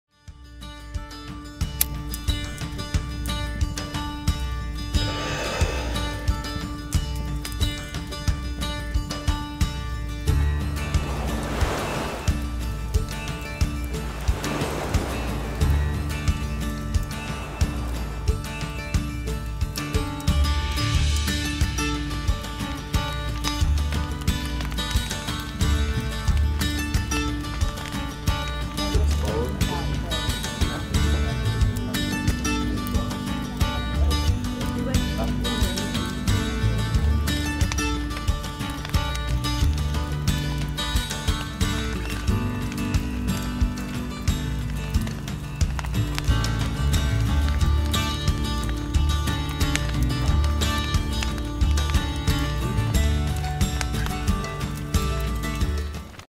Patagonia Campfire
Campfires bring life to expeditions.
Oscar Manguy expedition leader @nolspatagonia